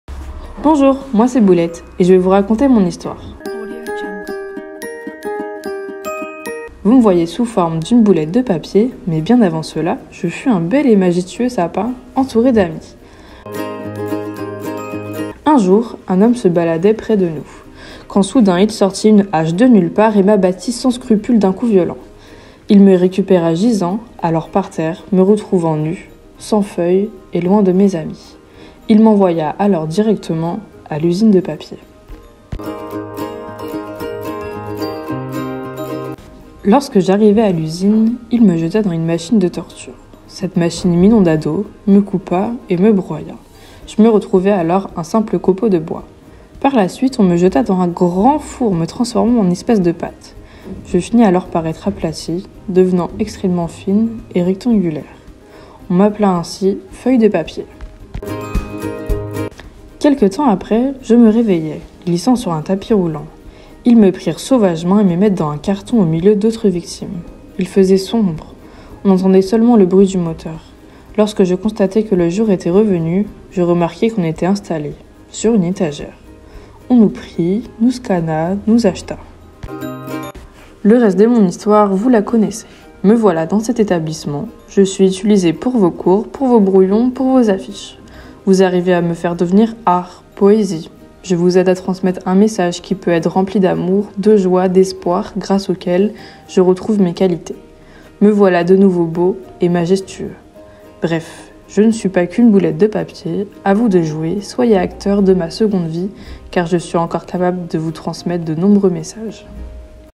L'histoire de Boulette
Découvrir l'incroyable histoire de boulette.
Réalisé par les BTS ESF 2 promo 2023 du Centre de Formation en Continue Jeanne Delanoue à Cholet.